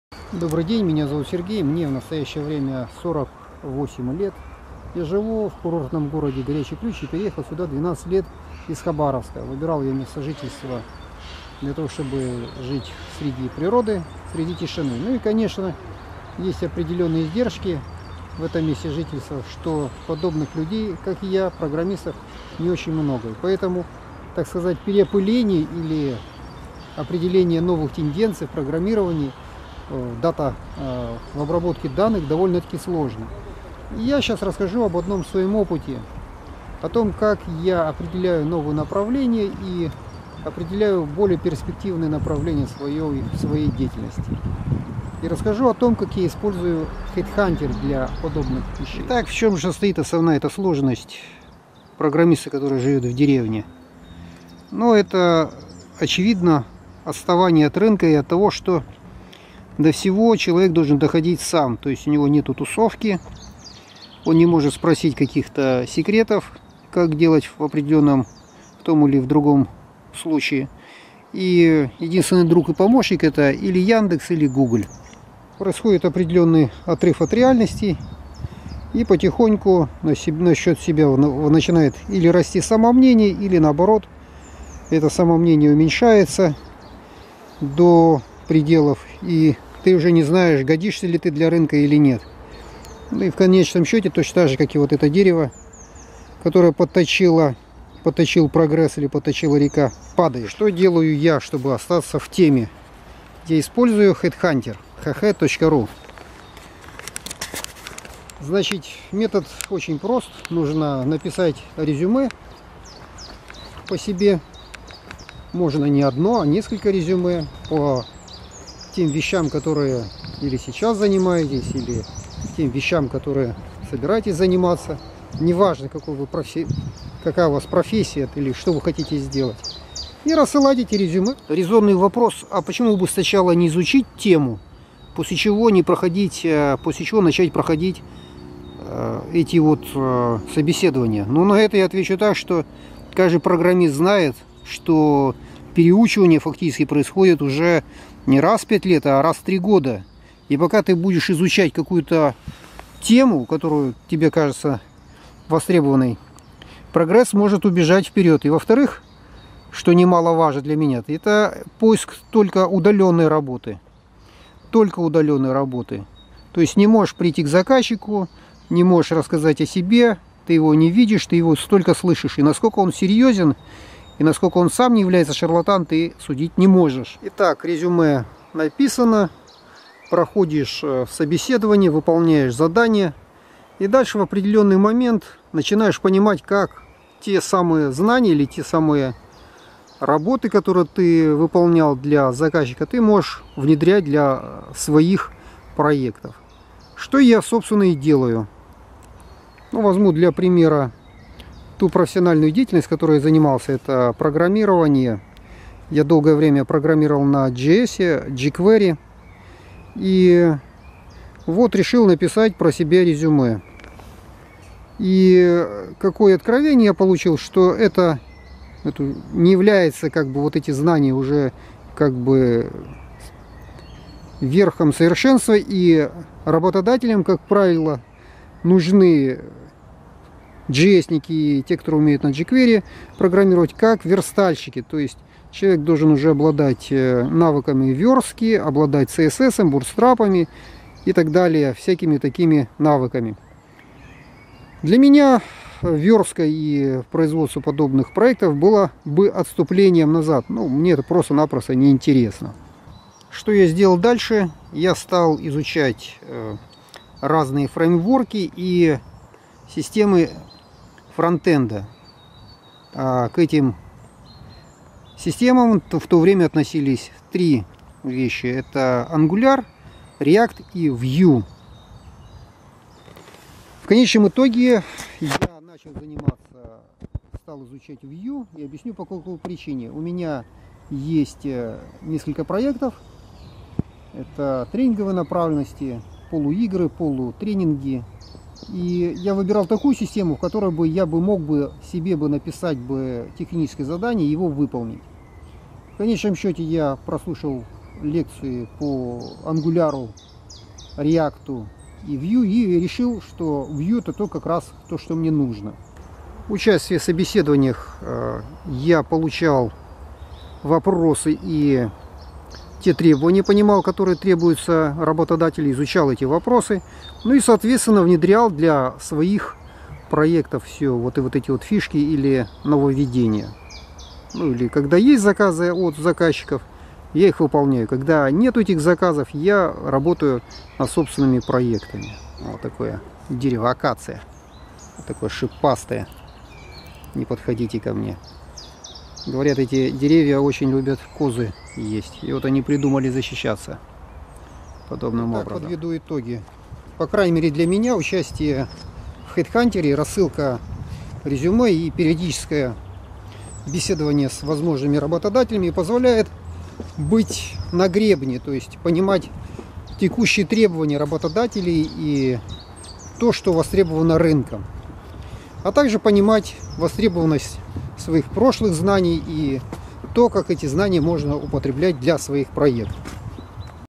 Тестирование рабочих навыков на бирже найма hh.ru
Как программисту жить в деревне и быть на гребне инноваций. Как не отстать от рынка.

В настоящее время мне 48 лет и я программист. программисткой деятельснотью занимаюсь со студенчества. Тогда это был очень сложный процесс. Я даже захватил перфокарты и белые халаты программистов. Потом были 90е годы. Потом изобретение интернета и в этот момент я решил возврватиться на рынок. Мне было тогда 27 лет. И, окружающие меня не понимали - ты же старик! восклицали все, кто слышал, что я купил книжки по ДЕльфи.